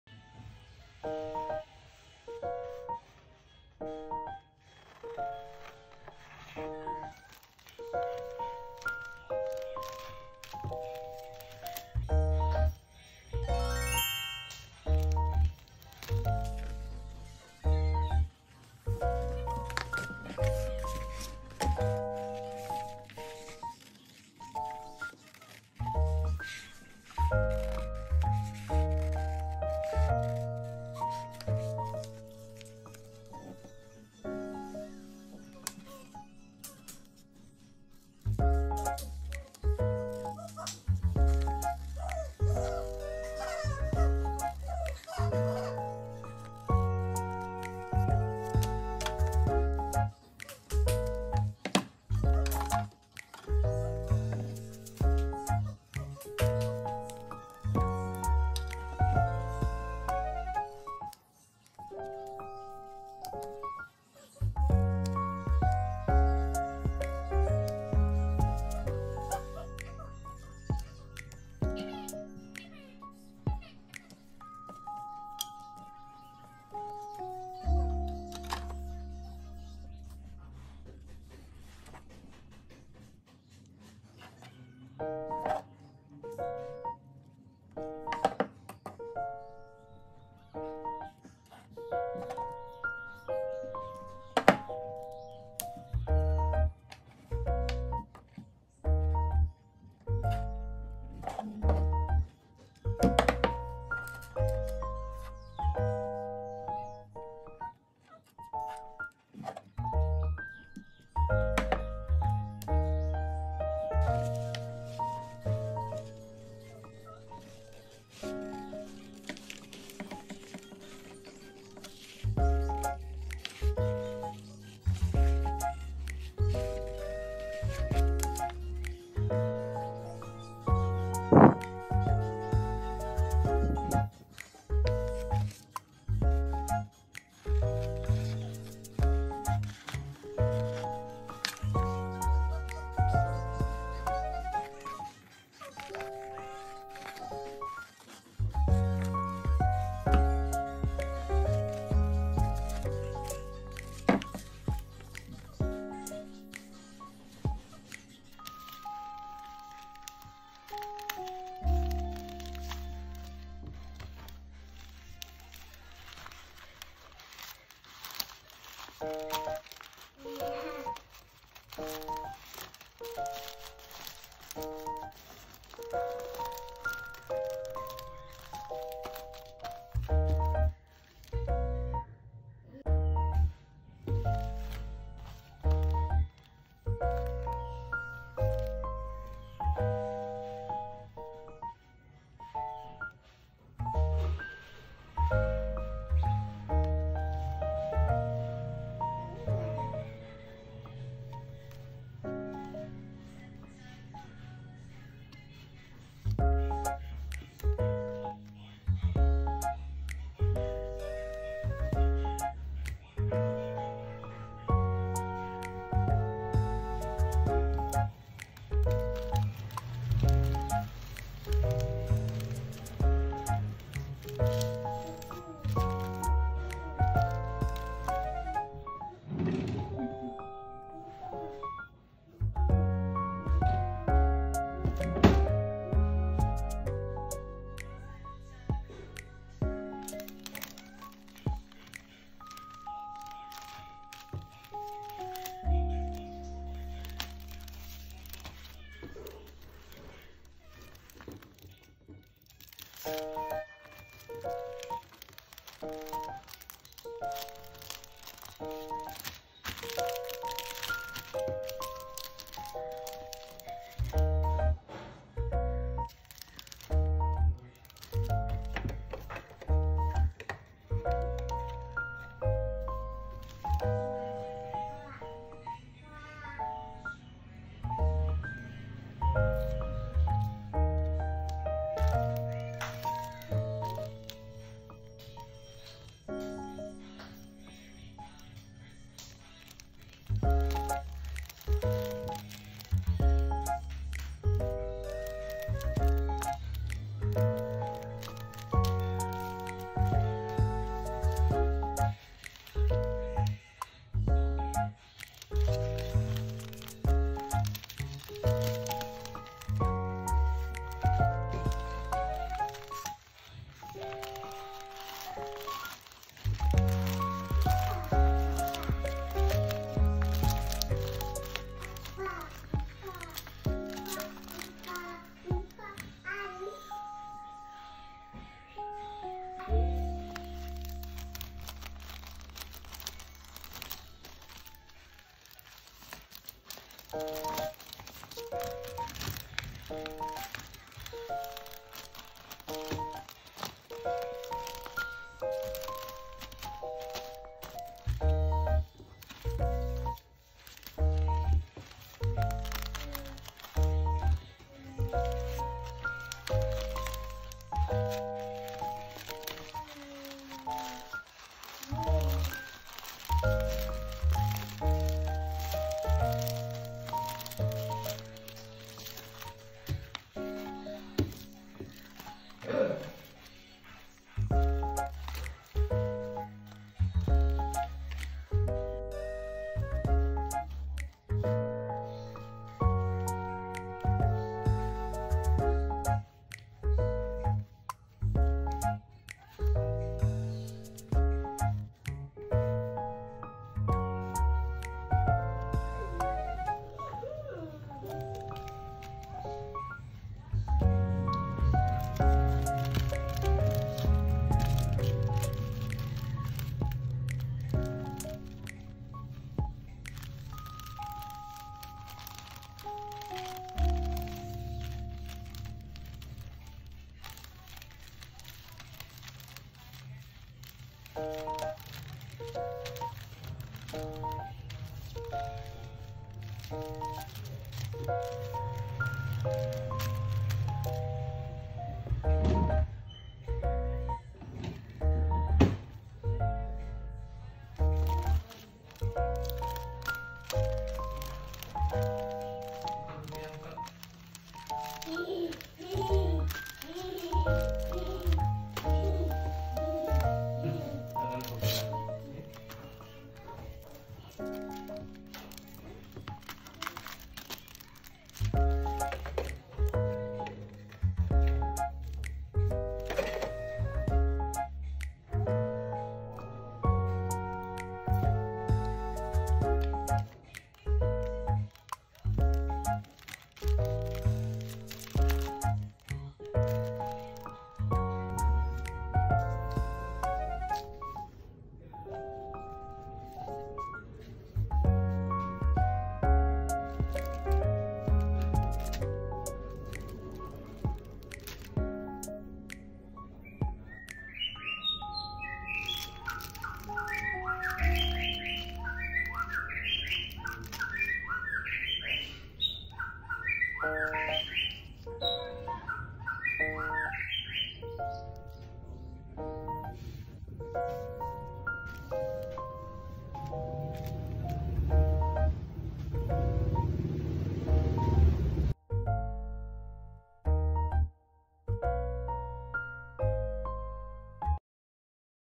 Studio Vlog | Preparing New Product Release Masker Duckbill, Pack Orders With Me | Indonesia
Halo teman-teman online,
Selamat datang kembali di saluran sederhana saya ini

Online shop instagram : @omegascarf.id
Shopee : omegascarf.id